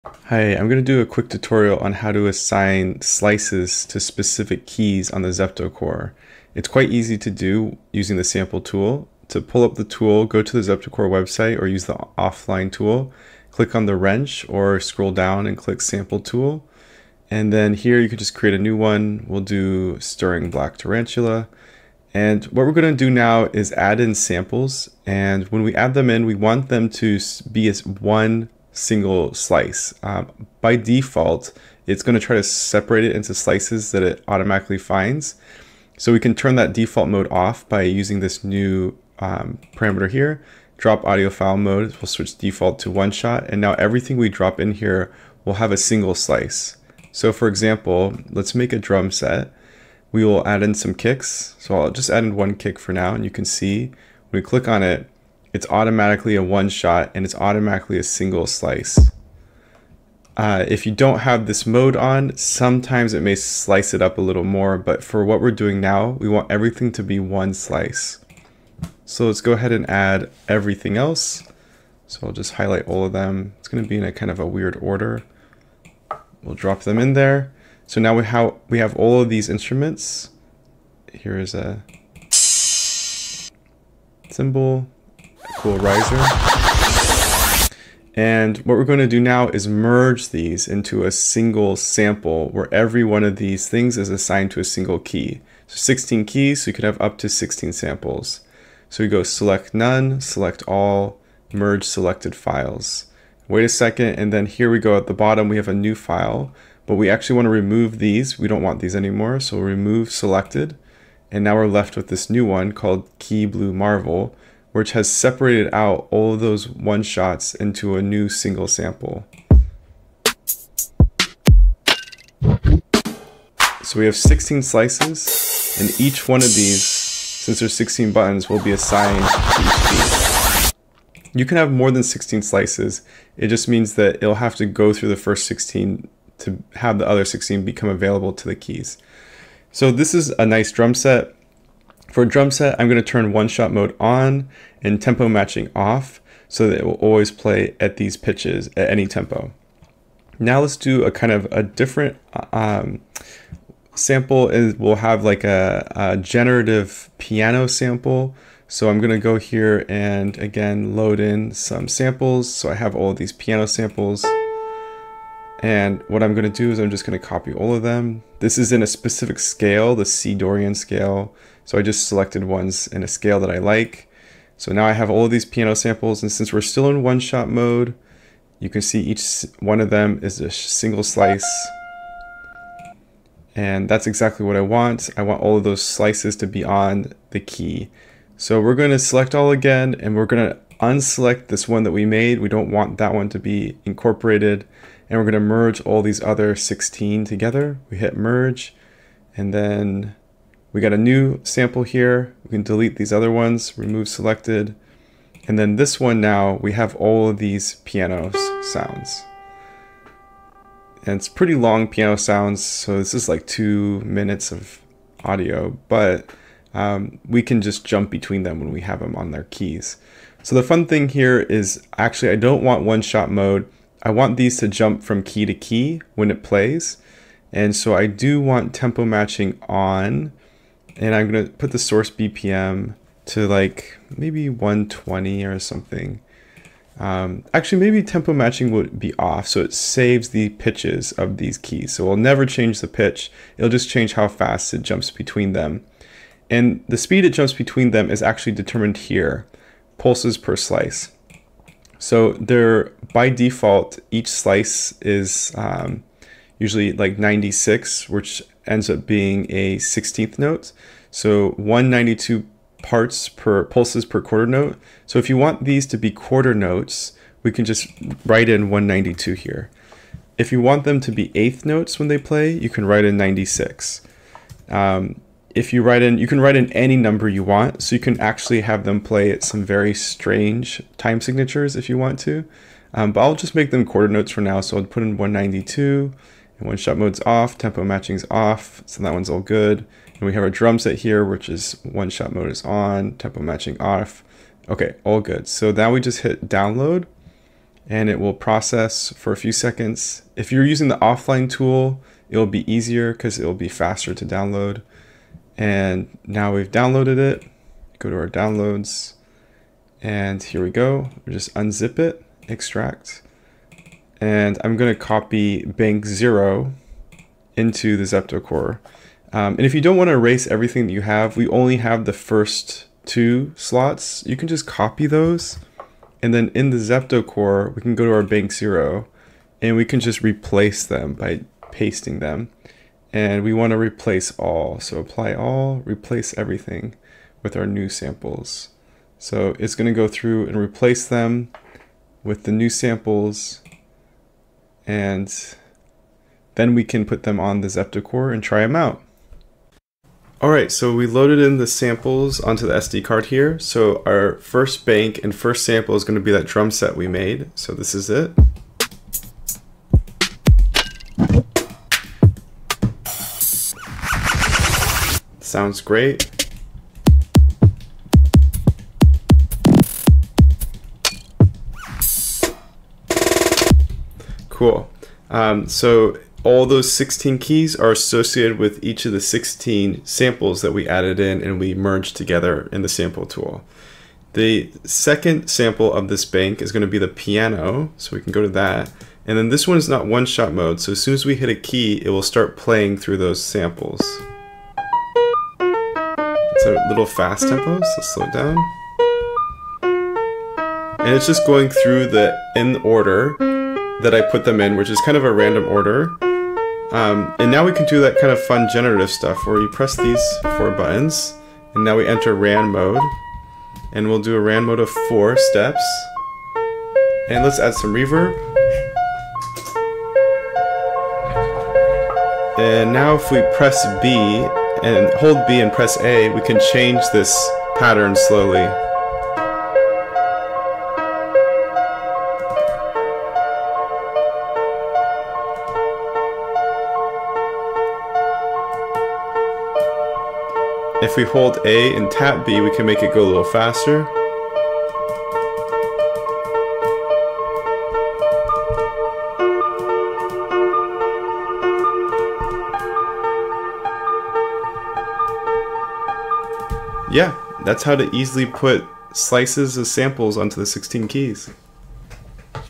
zeptocore tutorial: assigning slices to keys using the tool
the sample tool for zeptocore lets you easily create samples which single sounds assigned to specific keys.

00:00 introduction
00:32 switching to "oneshot" drop mode
00:47 lets make a drumset
02:35 lets make a generative piano sample
04:54 download to the sd card
05:53 lets try the drumset
06:26 lets try the piano and randomize it